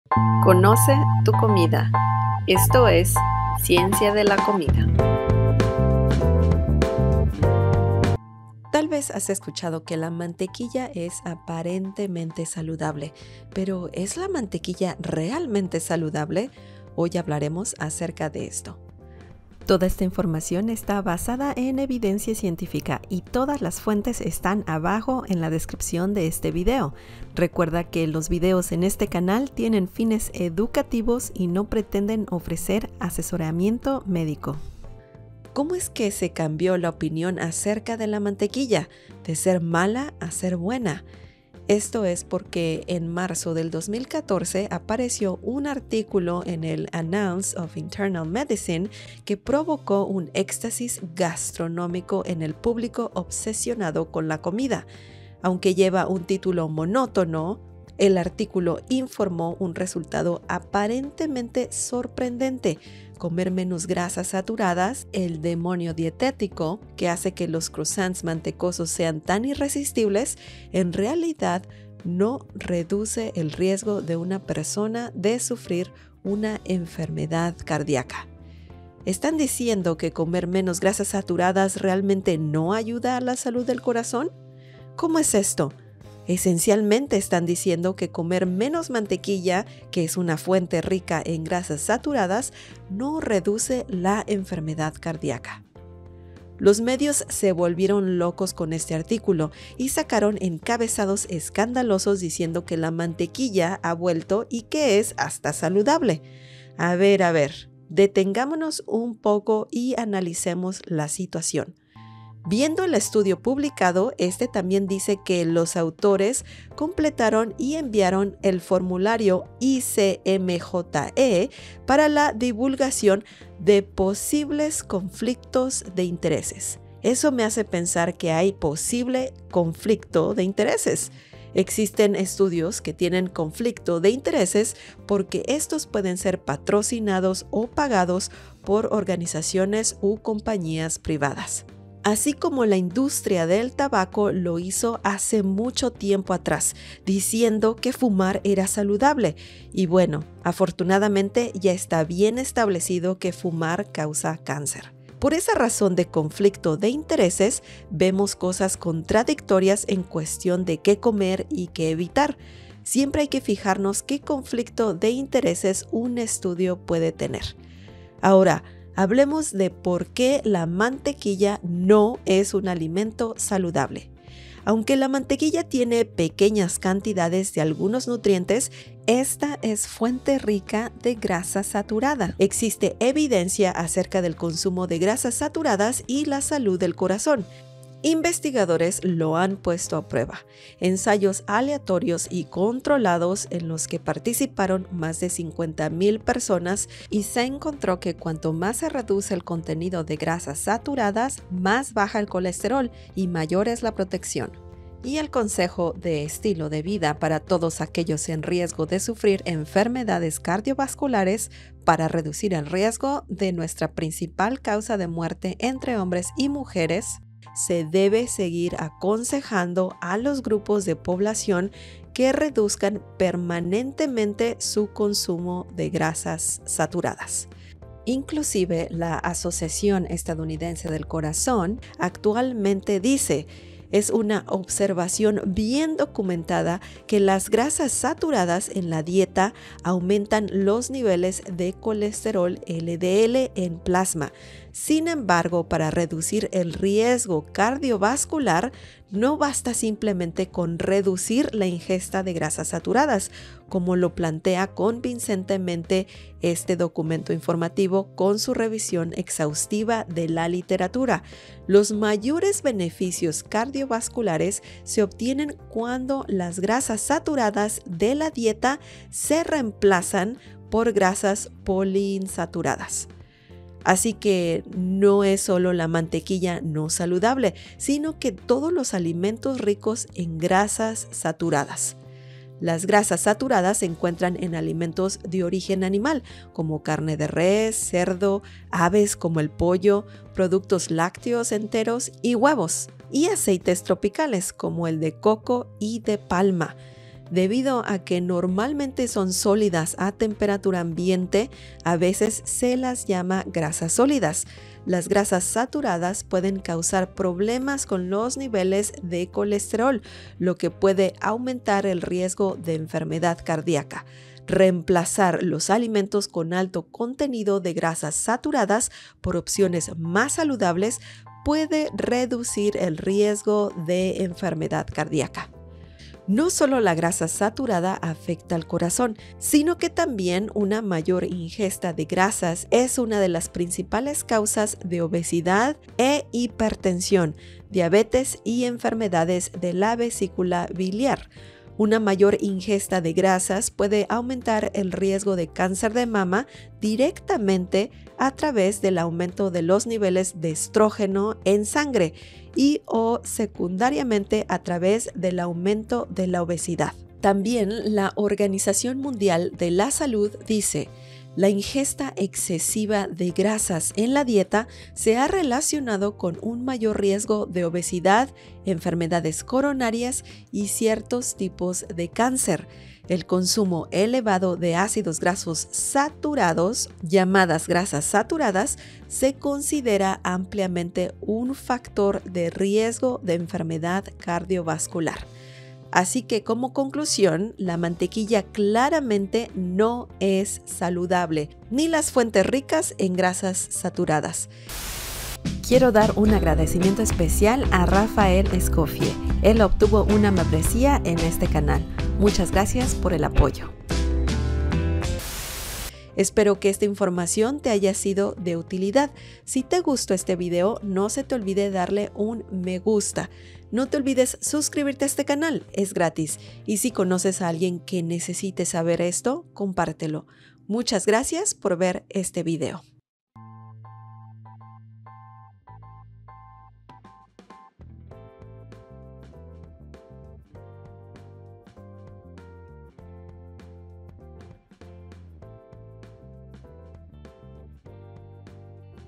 ¿ES LA MANTEQUILLA SALUDABLE? ¿OFRECE BENEFICIOS? | Ciencia de la Comida
Tal vez has escuchado que la mantequilla es aparentemente saludable. Pero, ¿es la mantequilla realmente saludable? Hoy hablaremos acerca de esto.